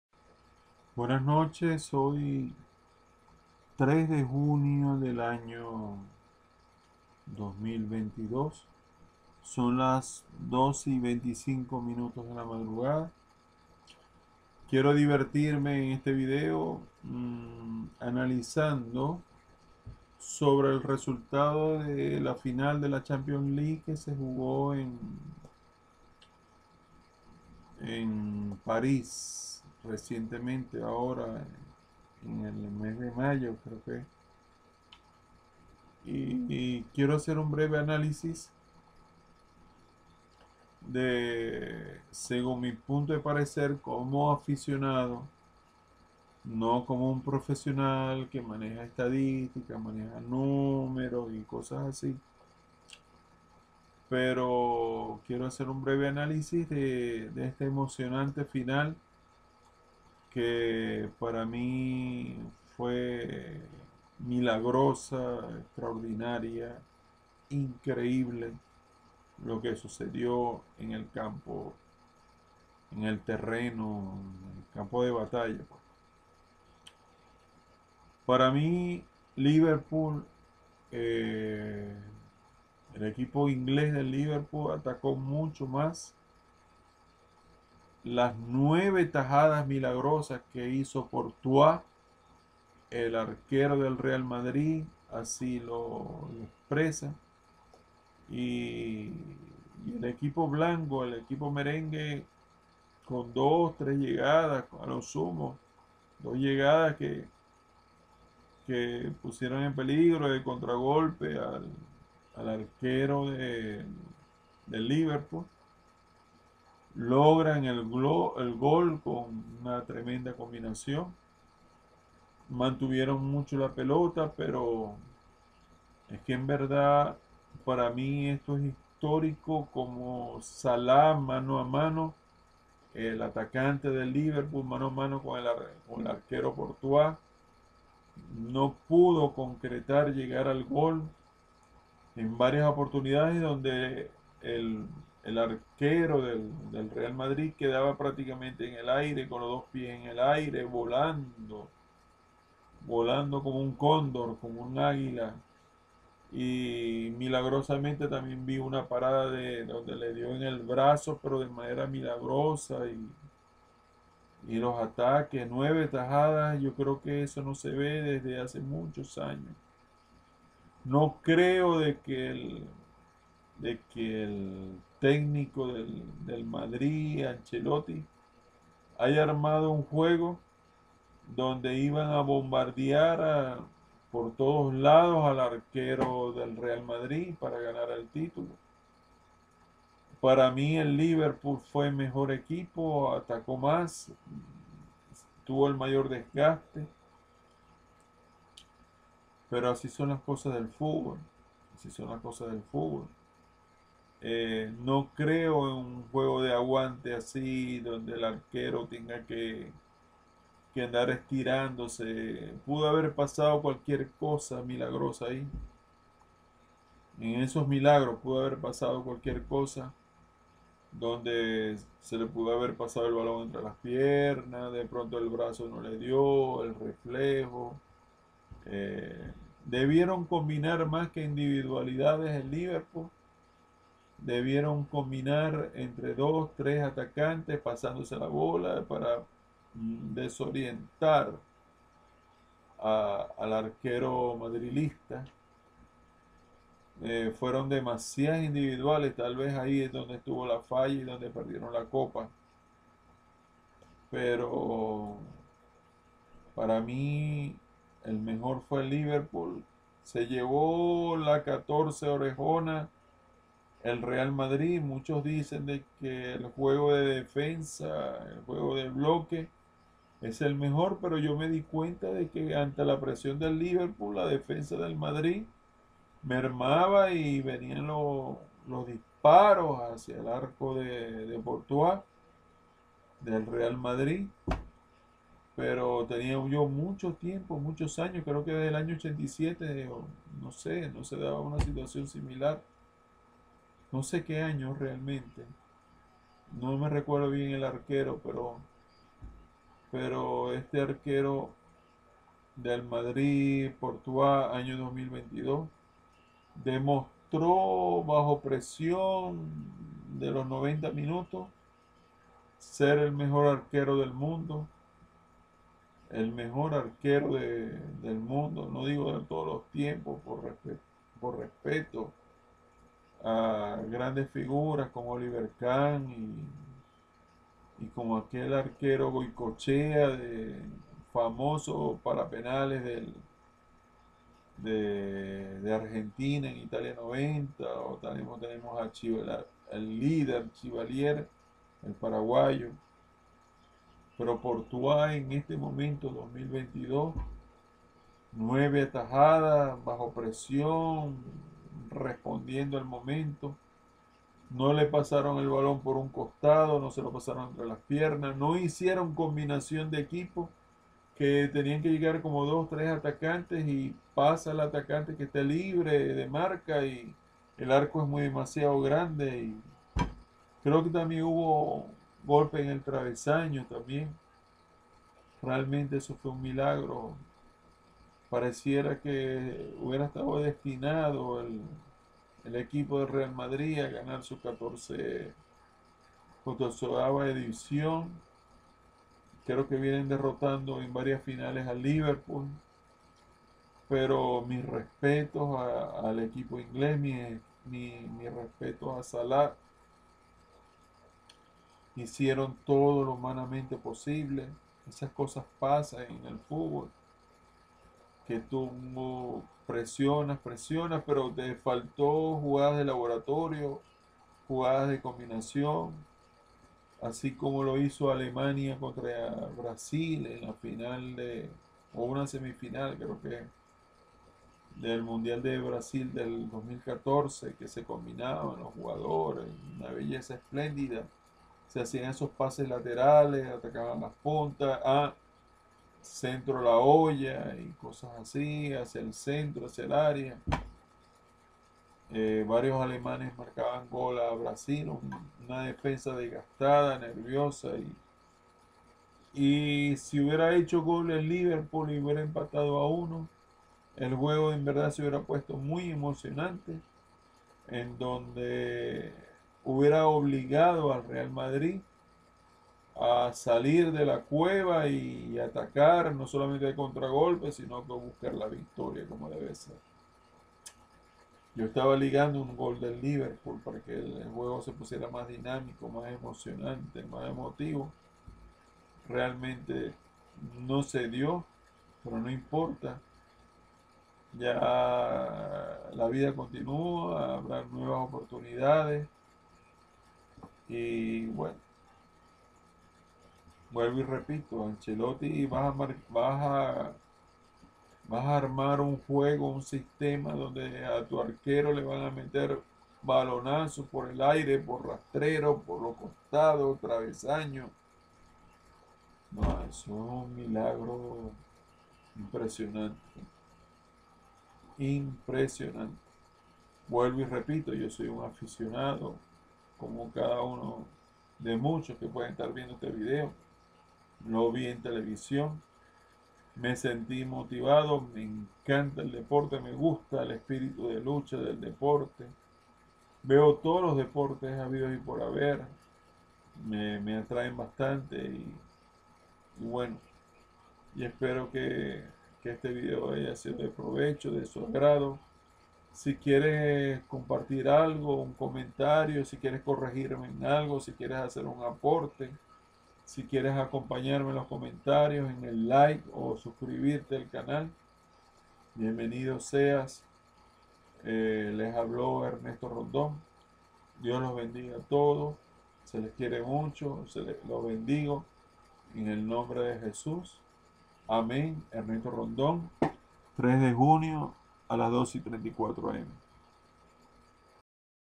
Final champions, Liverpool, Real Madrid, 2022....análisis detallado sobre esta final,
hablemos de futbol, de la final de la liga de campeones Liverpool real Madrid en parís año 2022. será posible que el entrenador del real Madrid, planifico un encuentro en donde su arquero, al borde del abismo realizo 9 atajadas salvando a su equipo. fue increíble, incluso saltando el arquero Portua por los aires, corriendo despavorido de un poste a otro para tapar el ataque del egipcio Salah parando goles con el antebrazo de carambola. esto fue increíble. Falto sangre fría para planificar batir este arquero en donde la defensa de el real Madrid hacia aguas ante la presión de quien fue en mi opinión el mejor equipo de la champions el Liverpool de Inglaterra.